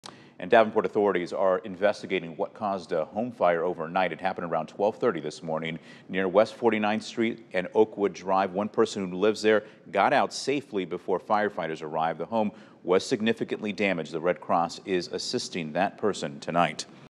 Davenport firefighters investigating overnight house fire
No one was injured in the blaze.